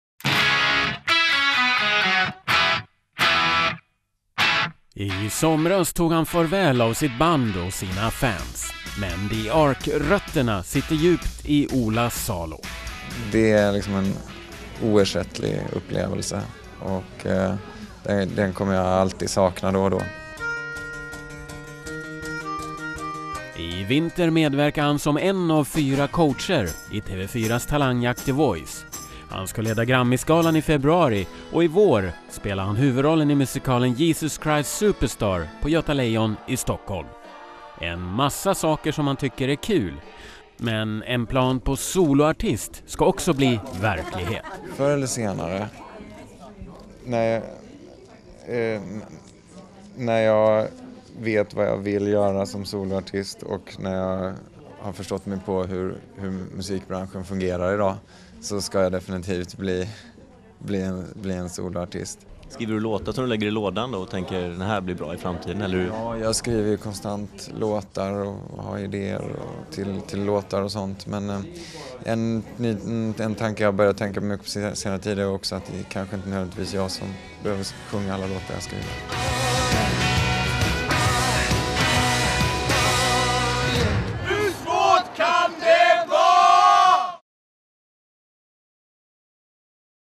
Salo drömmer solo
"Ola Salo planerar en solokarriär. Han skriver ständigt på nya låtar. Men först ska han vara coach, leda prisgala och spela Jesus. - När jag förstått hur musikbranschen fungerar i dag ska jag definitivt bli soloartist, säger han." [gp.se]